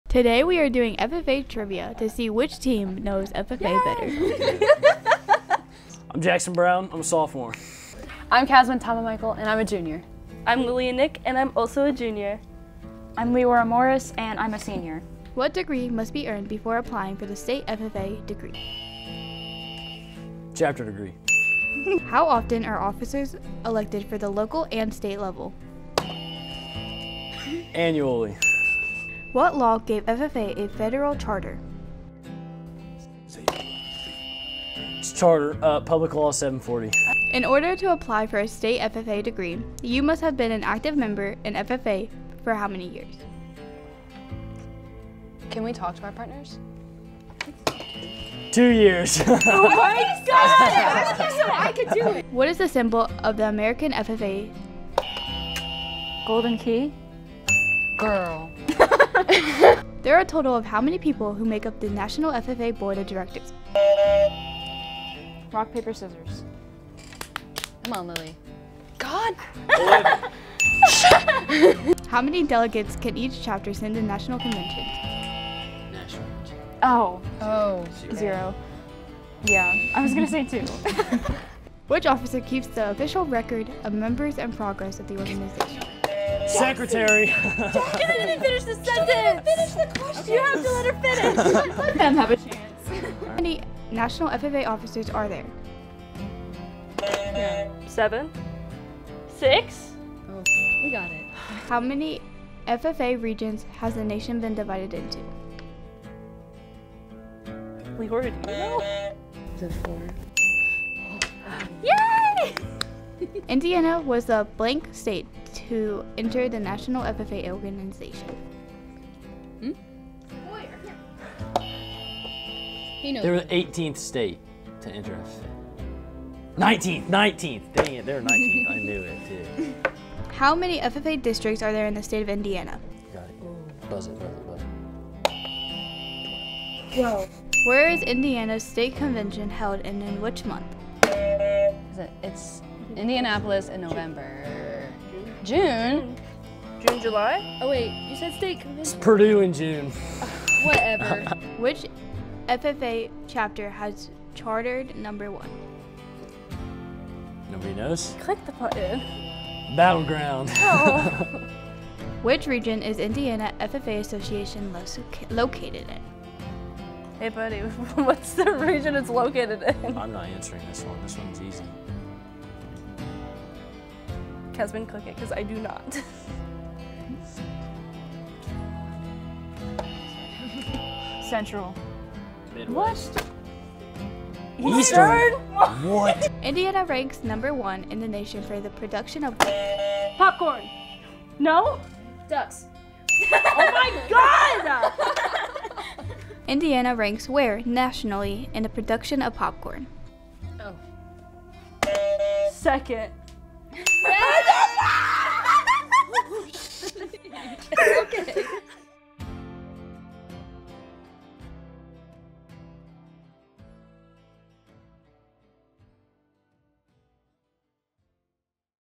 FFA Trivia
Four FFA members put their knowledge to the test to see how well they know their organization!

Credits: Sasha Linnehan, Regan Willis, Ari Sanchez, and Kenzy Blair